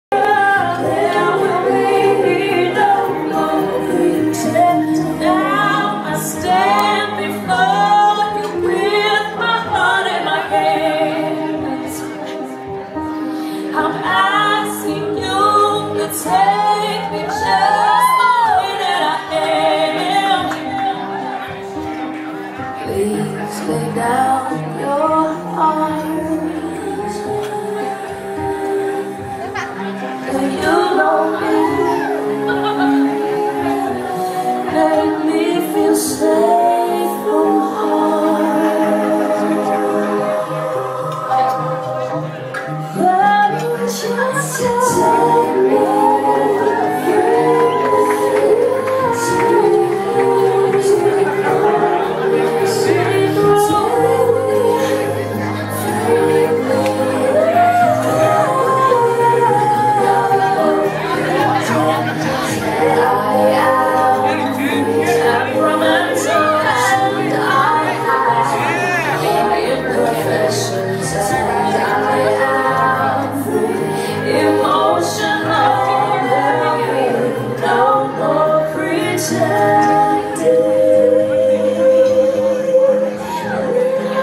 OC PRSA Protos Award Dancers
Yes, dancing at OC PRSA awards celebration in Laguna Beach, Calif.